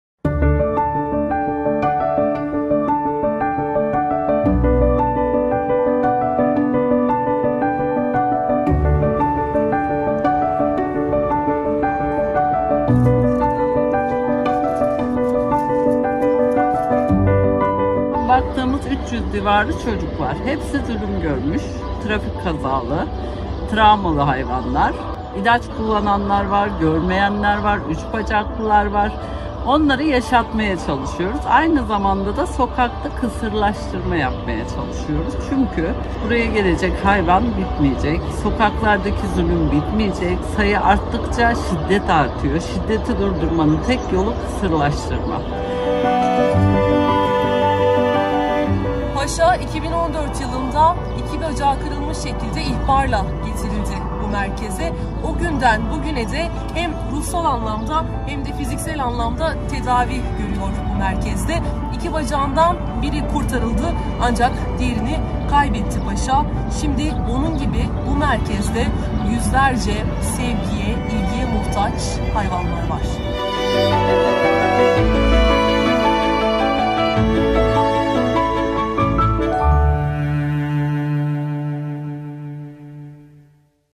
Yardıma muhtaç hayvanları iyileştirmeye çalışan çift
Kiminin gözü kiminin bacağı yok…

Zekiye ve Fahir Köklü çifti, 26 yıldır Ankara’da şiddet görmüş hayvanlara bakıyor. Yardıma muhtaç hayvanları iyileştirmeye çalışan çift, onlardan sevgisini ve merhametini eksik etmiyor.